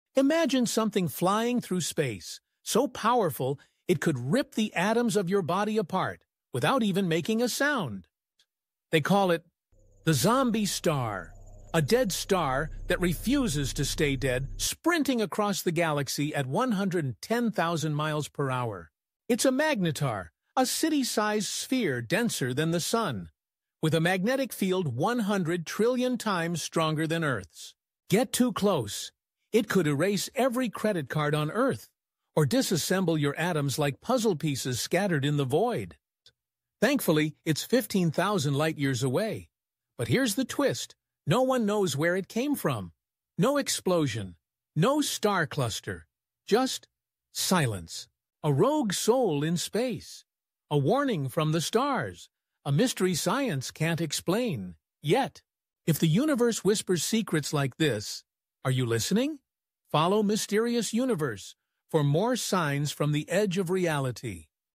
"The Zombie Star That Could Tear Earth Apart – And It's Real" | Mysterious Universe
There’s something racing through our galaxy—faster than any rocket ever built. It’s not alive, not dead… but if it came near Earth, it could destroy everything with its magnetic field. Meet the zombie star—a magnetar so powerful, it could tear your atoms apart without a sound. Where did it come from? No one knows. But it’s out there...